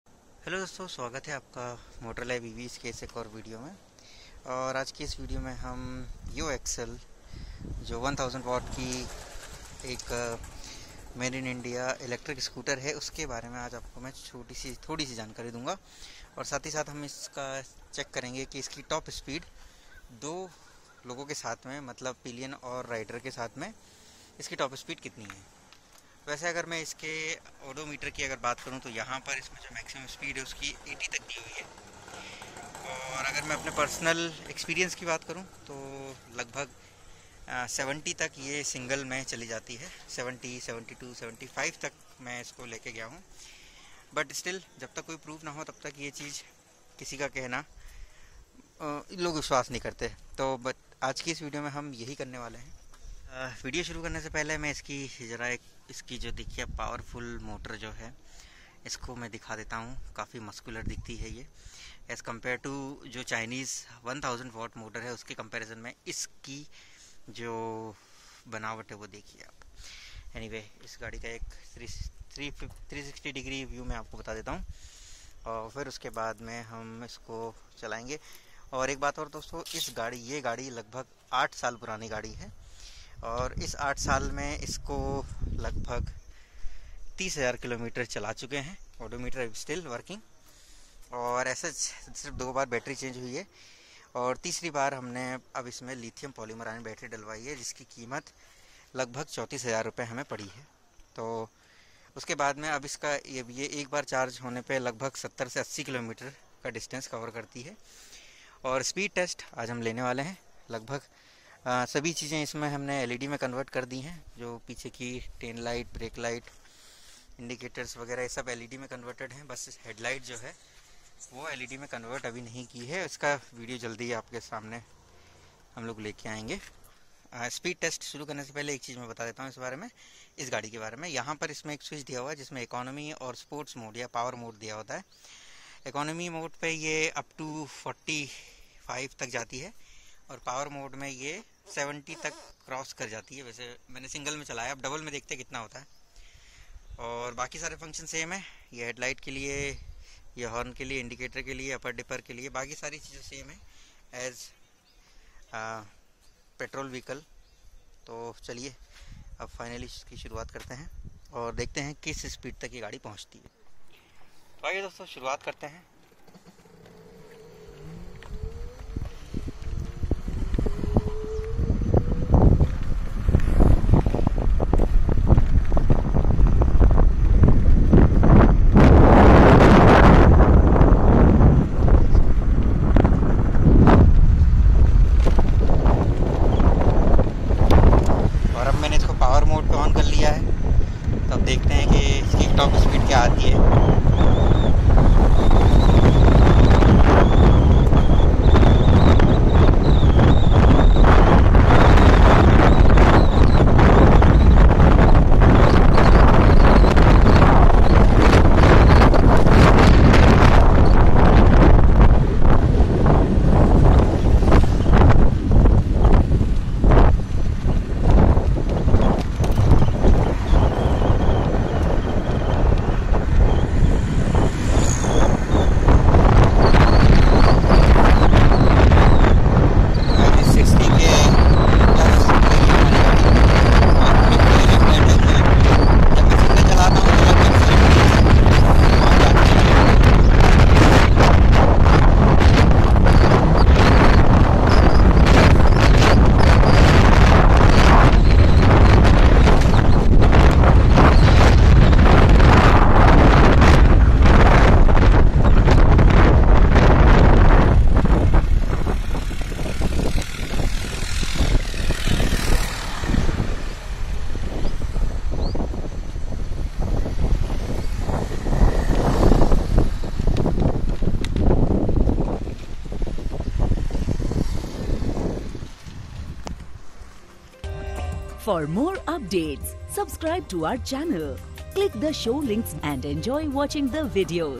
Top Speed II Yo ExL II Yo Byke Electric Scooter
In This video, I am going to Test Top Speed of Yo ExL Electric Scooter with Rider and Pillion, i hope this will help you to understand more about your Loved Electric Vehicle. if you still have any doubt or query you can call us via the callme4 app with ID "gr8bhagwan@cm4"
Jai Hind, Vandy Matrem